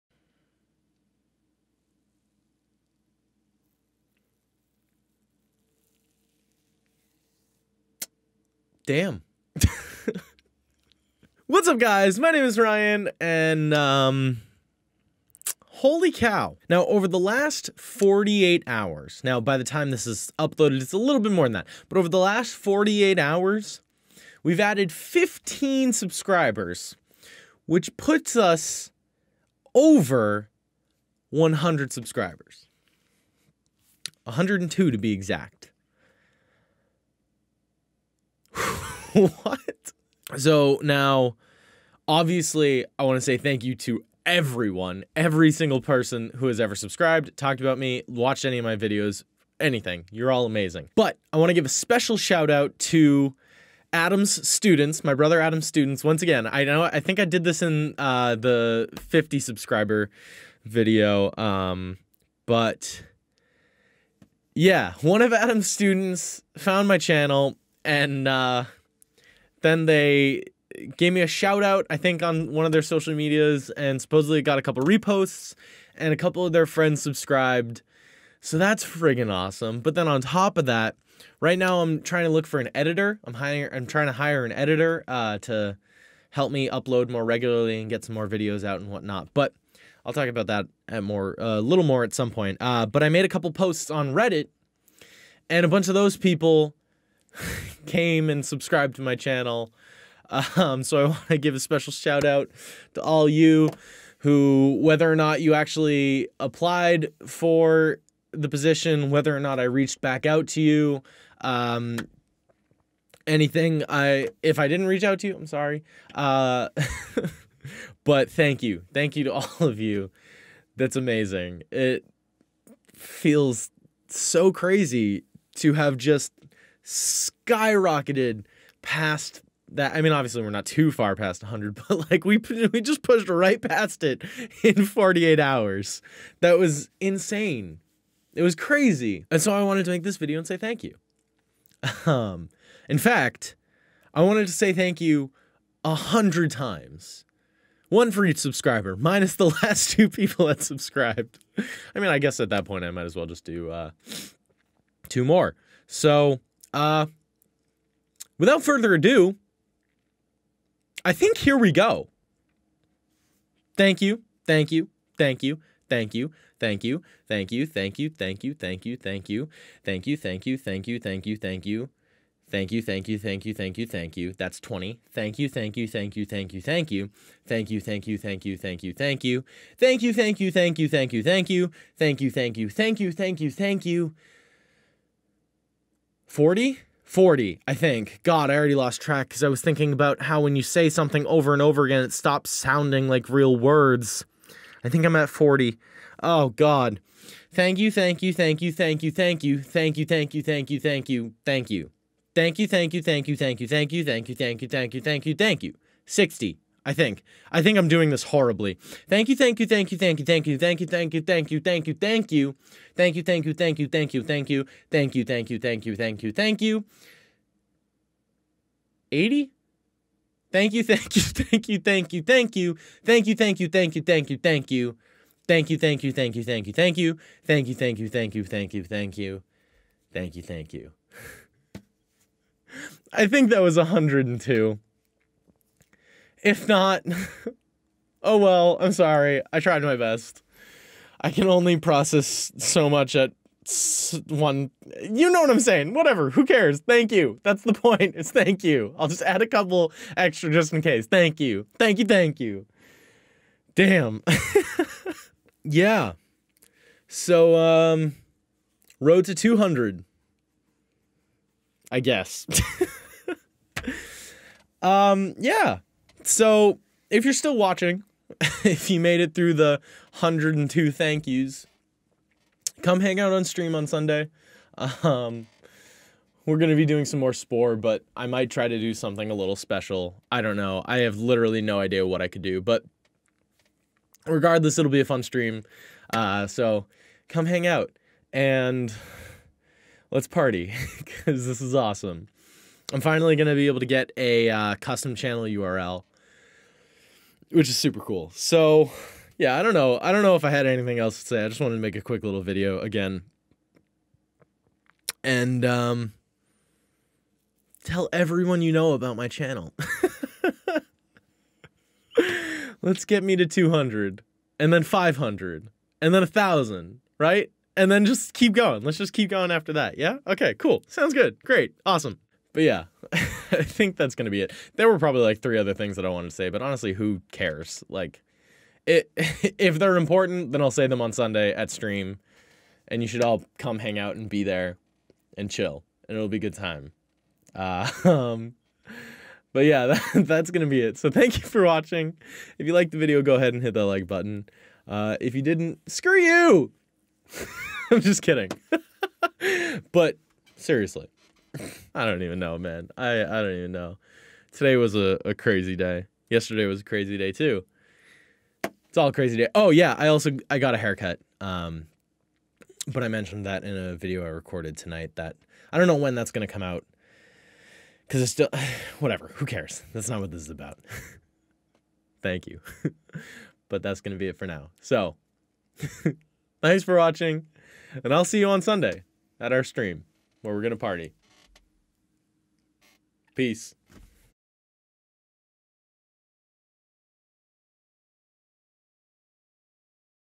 Saying 'Thank You' 100 Times for 100 Subscribers! You Guys Are Nuts!
I wanted to say thank you... 100 times... or more specifically 102 times. Let's get to 200!

My Gear:
Camera (Sony a5100)
Mic (Elgato Wave 3)
Camera Capture Card ($16) (Only used when streaming)
Game Capture Card
Sony Camera Power Supply
Monitor 1
Monitor 2
Microphone Arm
Camera Stand System -